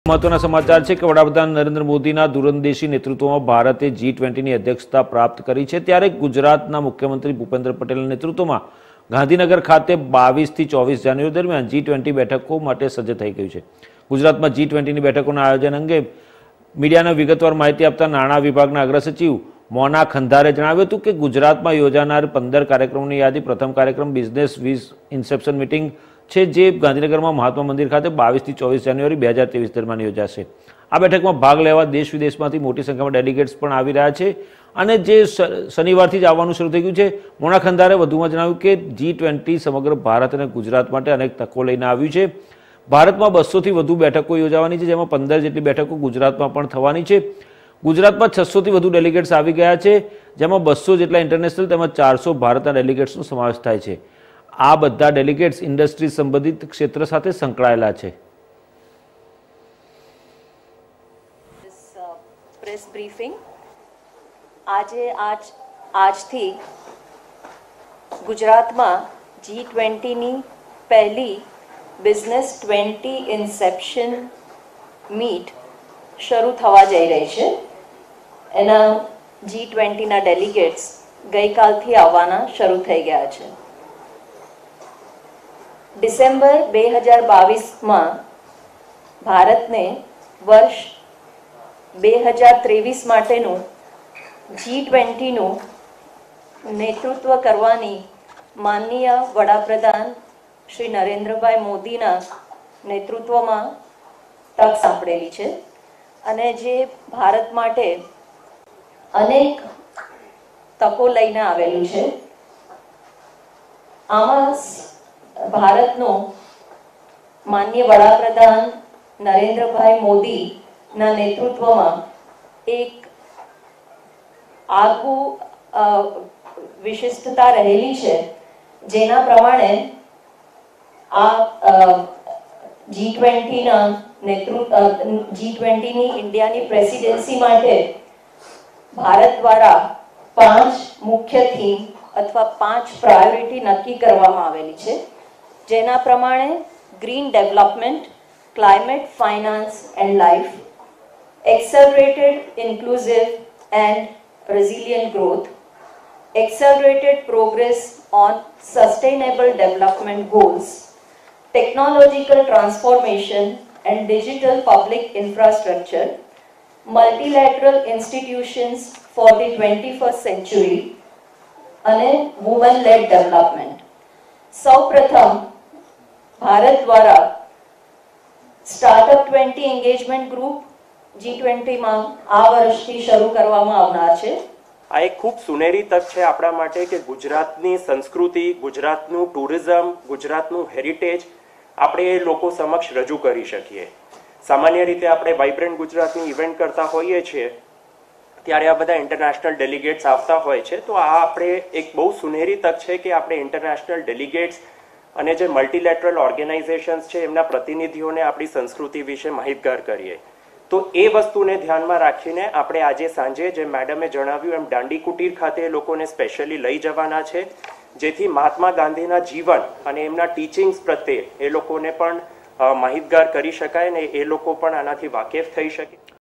B20 Inception Meeting Begins : ગાંધીનગરમાં B20 ઇન્સેપ્શન મીટીંગનો પ્રારંભ - ગુજરાતી સમાચાર
વડાપ્રધાન શ્રી નરેન્દ્ર મોદીના દૂરદર્શી નેતૃત્વમાં ભારતે G-20ની અધ્યક્ષતા પ્રાપ્ત કરી છે. ત્યારે ગુજરાત મુખ્યમંત્રી શ્રી ભૂપેન્દ્ર પટેલના નેતૃત્વમાં ગાંધીનગર ખાતે 22 થી 24 જાન્યુઆરી દરમ્યાન જી-20 બેઠકો માટે સજજ્ છે. ગુજરાતમાં જી-20ની બેઠકોના આયોજન અંગે મીડિયાને વિગતવાર માહિતી આપતા નાણા વિભાગના અગ્ર સચિવ શ્રી મોના ખંધારે જણાવ્યું હતું કે, ગુજરાતમાં યોજાનાર 15 કાર્યક્રમોની યાદીમાં પ્રથમ કાર્યક્રમ બિઝનેસ 20 ઇન્સેપ્શન મીટિંગ છે, જે ગાંધીનગરમાં મહાત્મા મંદિર ખાતે 22 થી 24 જાન્યુઆરી, 2023 દરમિયાન યોજાશે. આ બેઠકમાં ભાગ લેવા દેશ-વિદેશમાંથી મોટી સંખ્યામાં ડેલિગેટ્સ પણ આવવાના ગઇકાલથી જ શરૂ થઇ ગયા છે.